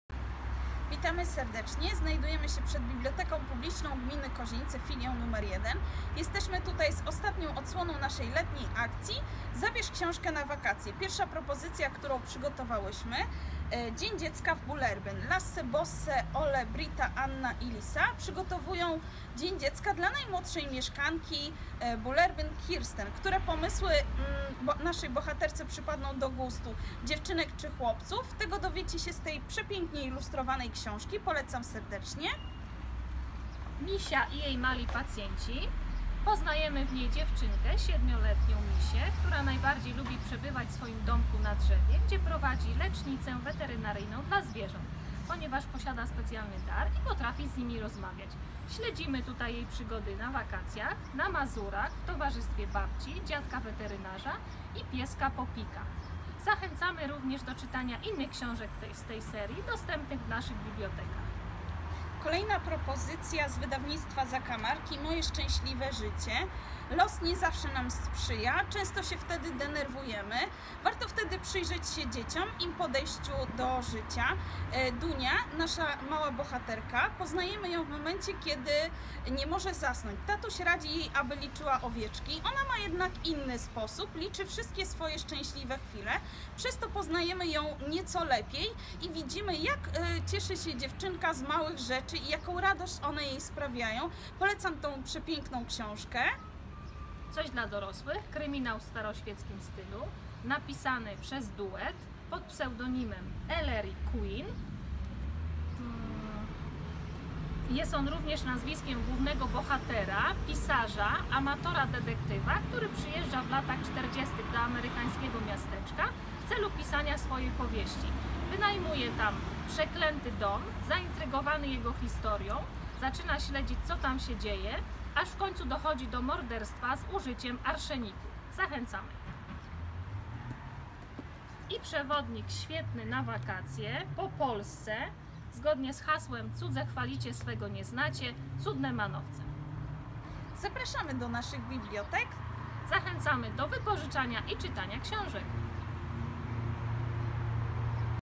Zakończenie letniej akcji w Filii nr 1 "Zabierz książkę na Wakacje"
Kończą się wakacje i nasza letnia akcja „Zabierz książkę na WAKACJE”.
W każdą środę polecaliśmy wyjątkowe książki, które miały za zadnie
sprawiać przyjemność i umilać wypoczynek. Mamy ogromną nadzieję, że skorzystaliście z naszych „polecajek” :) Oczywiście czekamy na WAS
w naszych bibliotekach przez cały rok ‼️
Serdecznie zapraszamy 😀

Książki, które polecamy w filmie:
Astrid Lindgren „Dzień Dziecka w Bullerbyn”
Aniela Cholewińska-Szkolik „Misia i jej mali pacjenci. Wyjazd na Mazury”
Rose Lagercrantz „Moje szczęśliwe życie ”
Queen Ellery „Przeklęte miasto”
Grzebyk Marta „Cudne manowce. Wszystko, co w Polsce najlepsze”